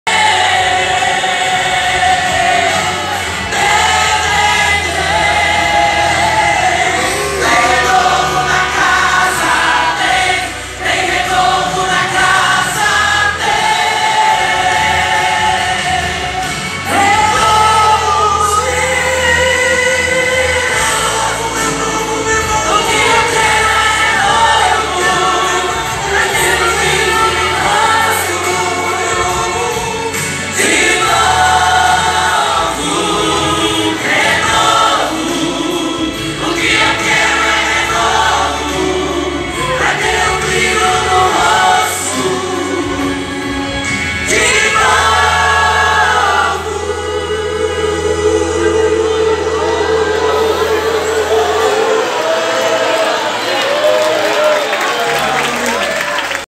TEM RENOVO NA CASA TEM AGO DA CEADER EM CAMPOS DOS GOITACAZES
MOMENTO DE LOUVOR E ADORAÇÃO NA AGO DA CEADER EM CAMPOS DOS GOITACAZES.